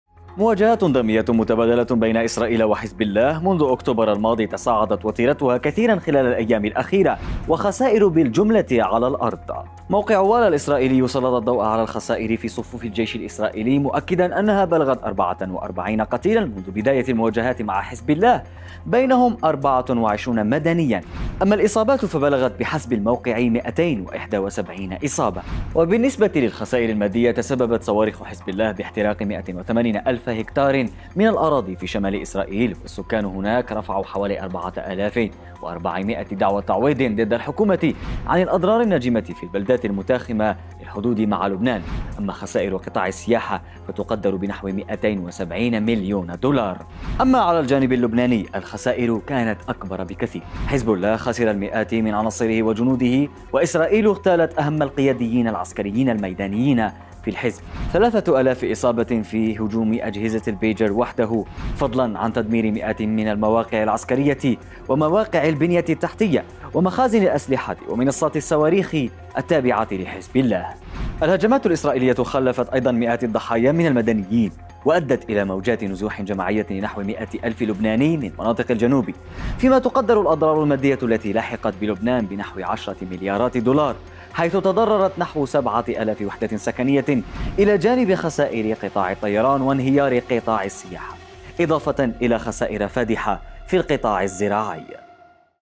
المواجهات بين "حزب الله" وإسرائيل.. فاتورة الخسائر حتى الآن
خسائر بشرية ومادية فادحة للمواجهات بين "حزب الله" وإسرائيل
لمعرفة أحدث المستجدات.. عبر البث المباشر من هنا 👇
وبإمكانكم متابعة العربية على منصات التواصل